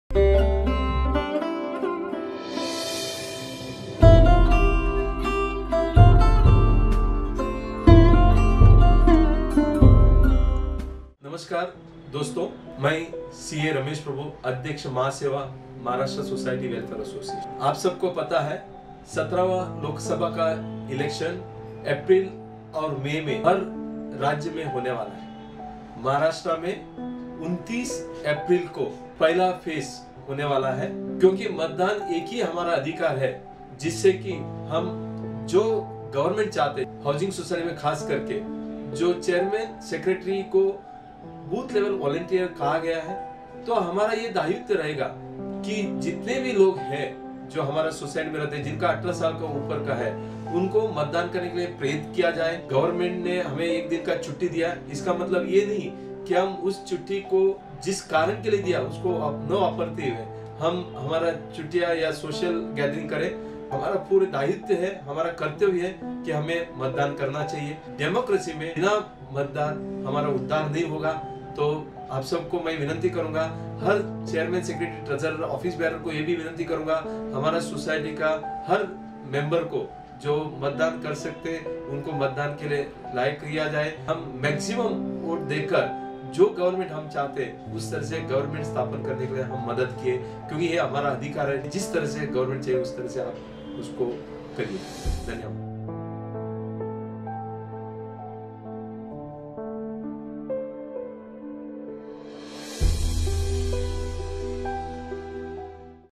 General Election 2019 (Hindi)
Presented by  Maharashtra Societies Welfare Association (MahaSeWA). Subject General Election 2019 (Hindi). by the keynote speaker is Housing Guru CA Ramesh Prabhu, Chairman, MahaSeWA.

Maharashtra Societies Welfare Association (MahaSeWA)
A-2/302, Laram Center, Opp. Rly Platform No. 6, Near Bus Depot, Andheri (W), Mumbai - 400 058.